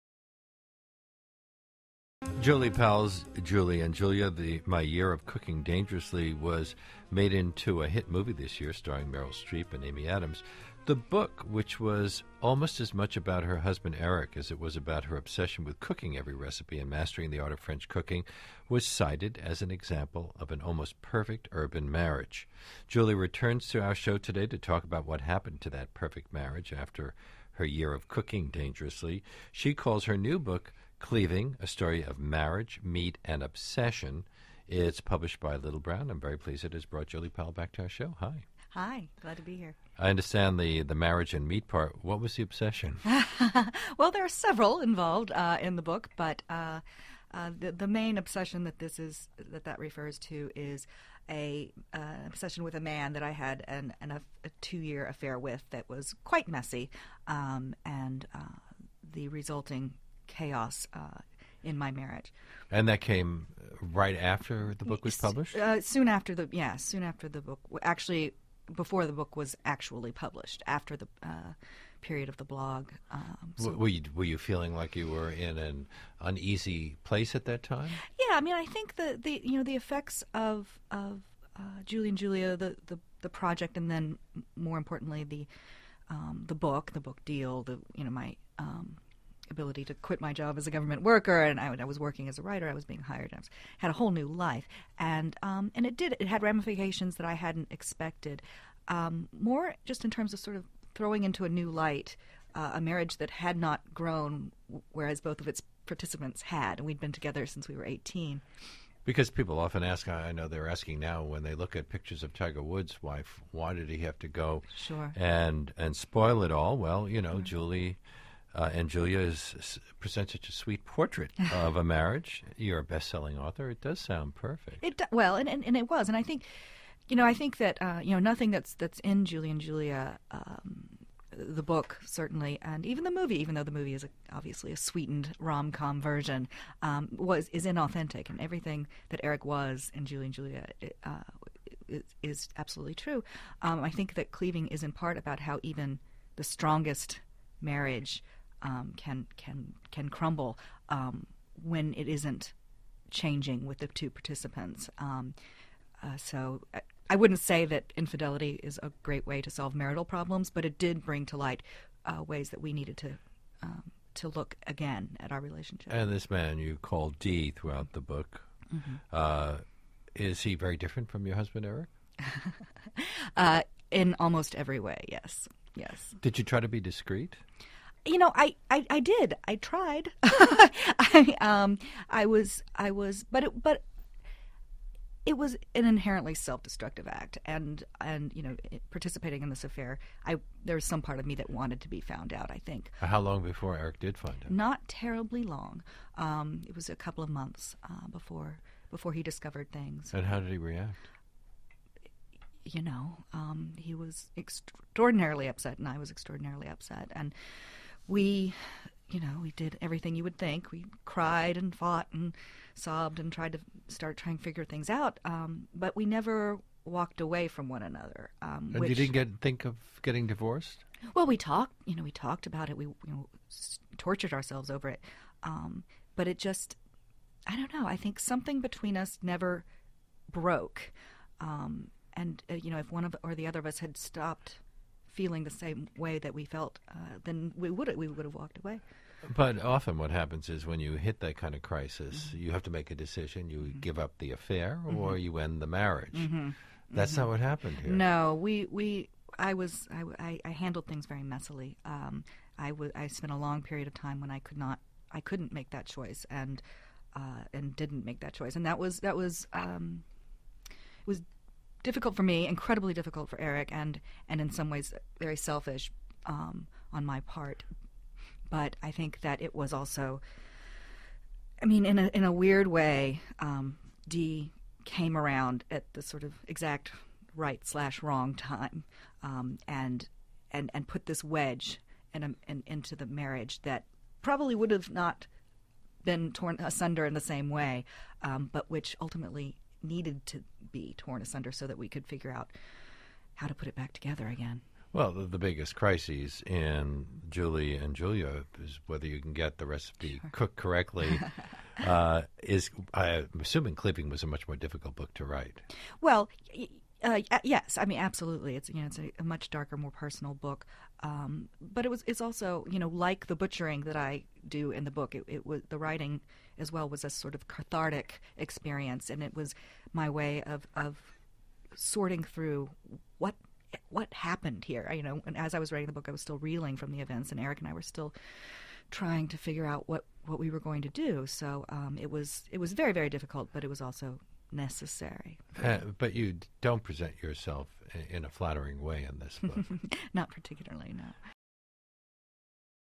Julie Powell on Having an Affair
Listen to the whole interview:
Julie Powell talks about learning the art of butchering and traveling from Argentina to Ukraine to Japan to learn about meat. Her new memoir is Cleaving: A Memoir of Marriage, Meat, and Obsession.